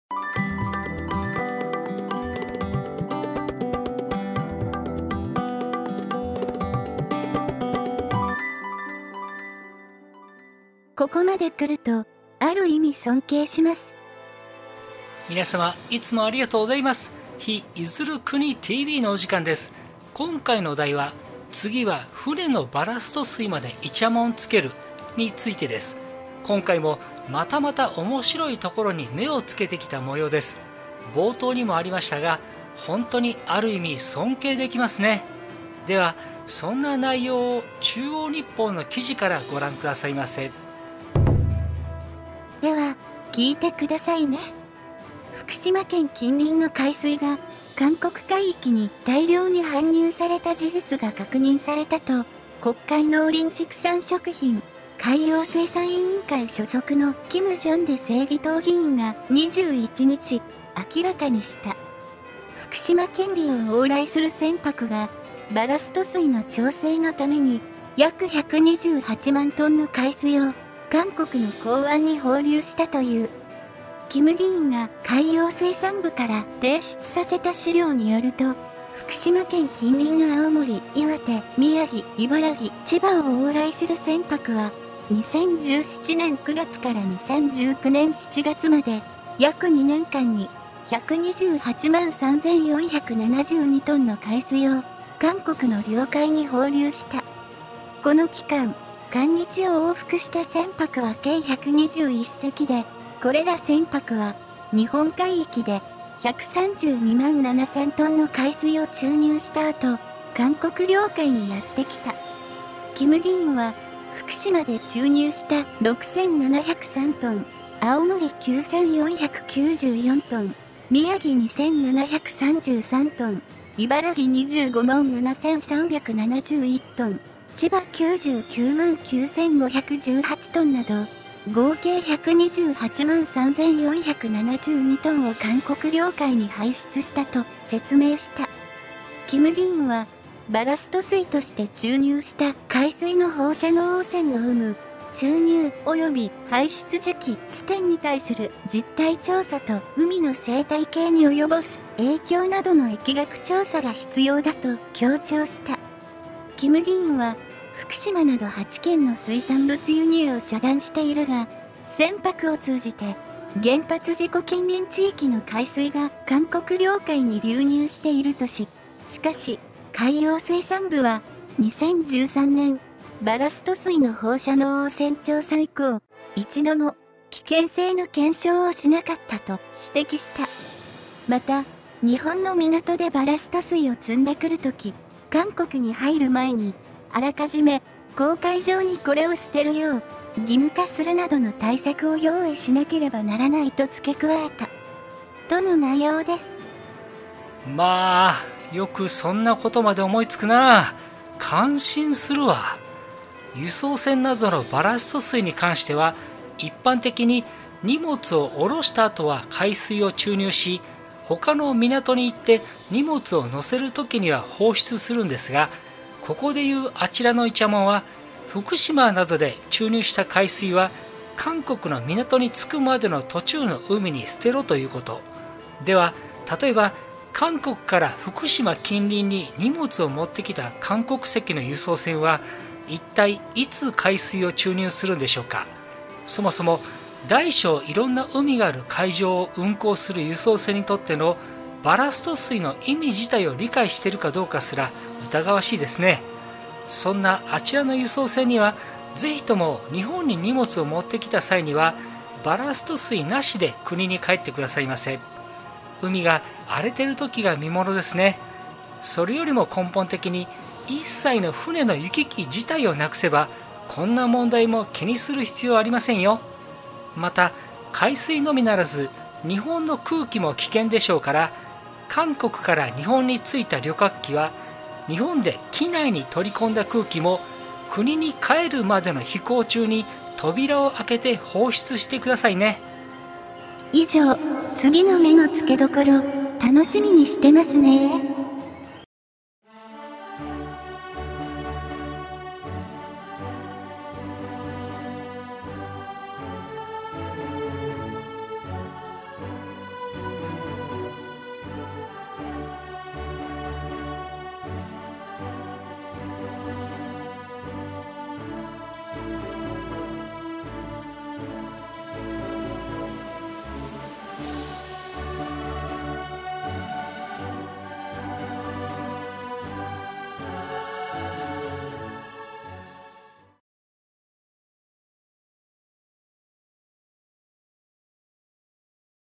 韓国がまた日本の放射能？について驚きの指摘「船のバラスト水が…」自国の船はどうするの？【日出づる国TV】
Twitterはこちらです！　日出づる国TV　@TV94664920